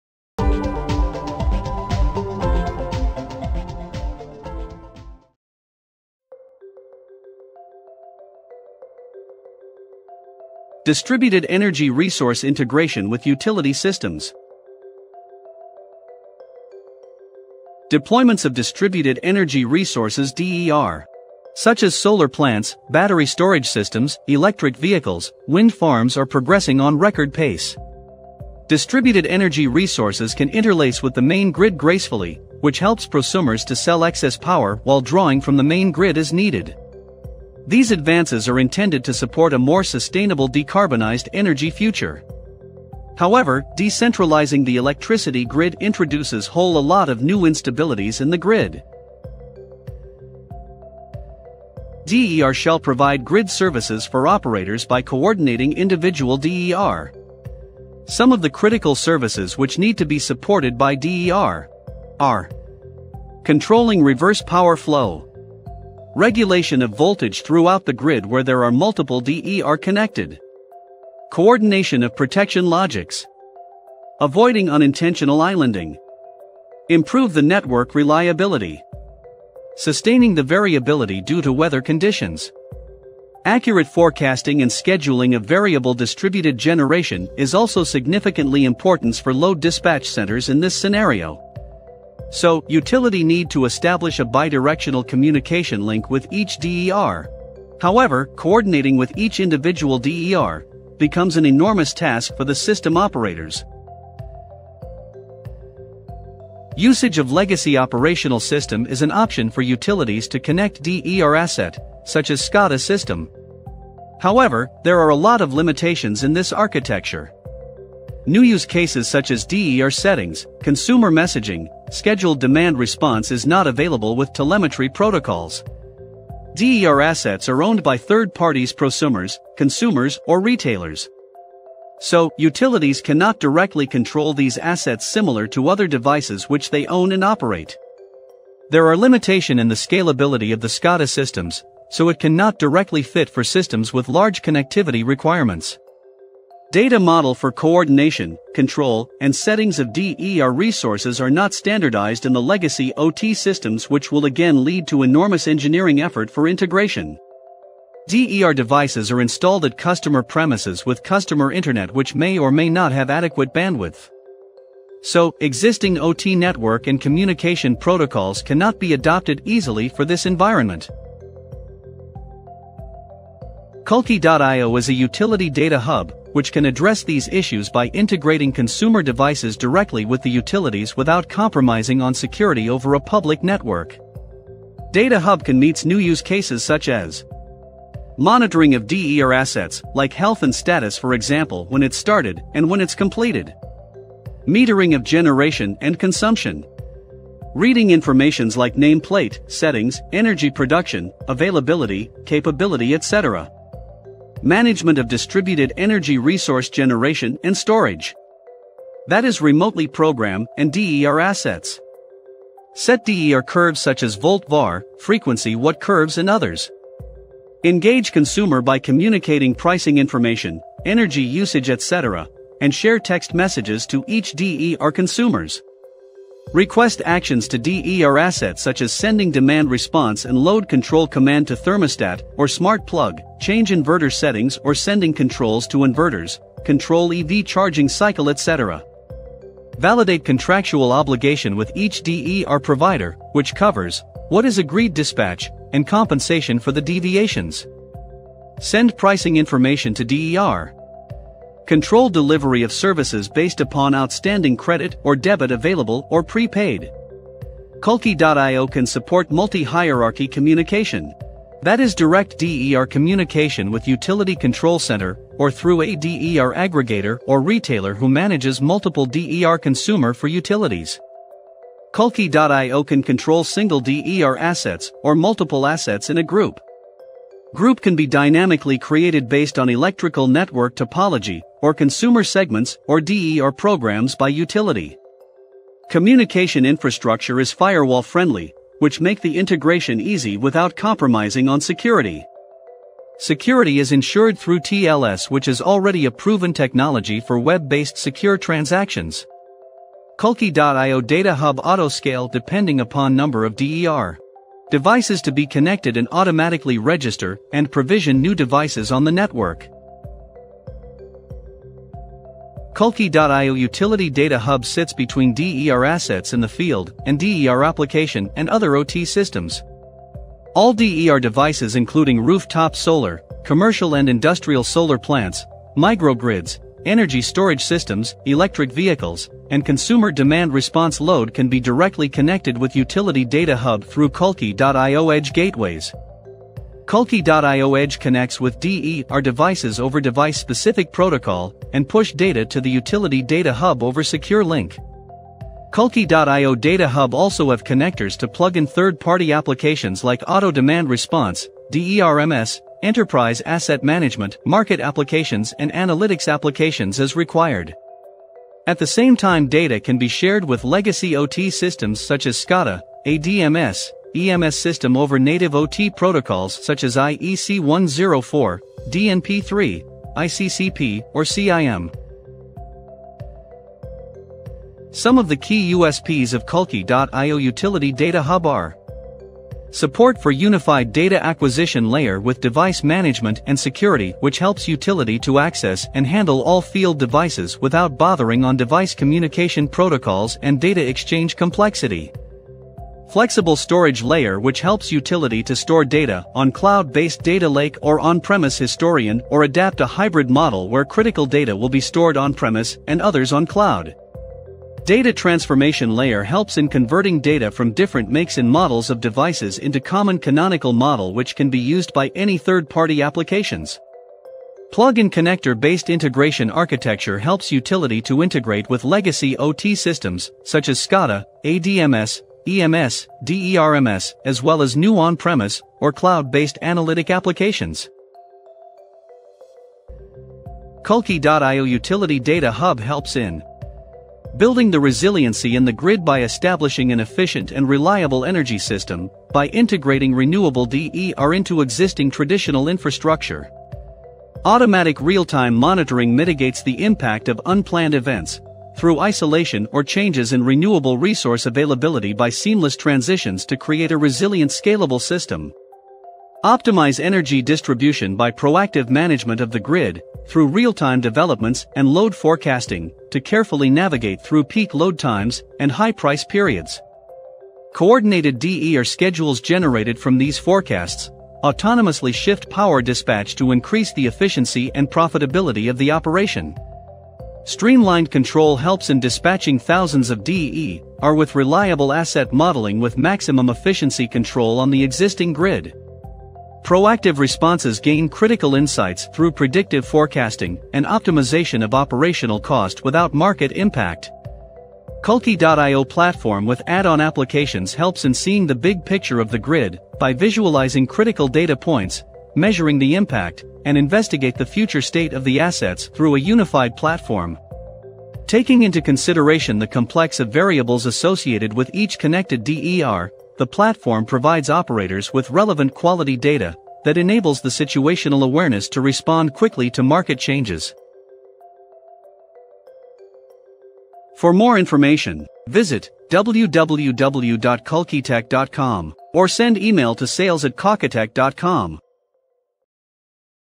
Kalki io DER Data Hub | DER Aggregator | DER Gateways | Utilities | IoT Solutions for Utilities
Deployments of Distributed Energy Resources such as solar plants, battery storage systems (BSS), electric vehicles (EV) etc are progressing on record pace. While DER can interlace with the main grid gracefully, it throws up huge challenges for utilities to integrate and manage them gracefully.

Explore how Kalki.io DER Data Hub helps utilities to build Resiliency in the Grid by integrating renewable DERs into existing traditional infrastructure. Optimize Energy distribution by proactive management of the grid, through real-time developments and load forecasting, to carefully navigate through peak load times, and high price periods. Coordinate DER schedules generated from these forecasts, autonomously shift power dispatch to increase the efficiency and profitability of the operation. DER data hub helps in streamlining the dispatch of thousands of DERs with reliable asset modeling with maximum efficiency control on the existing grid.

Kalki.io platform with add-on applications helps in seeing the Big Picture of the grid, by visualizing critical data points, measuring the impact, and investigate the future state of the assets through a unified platform.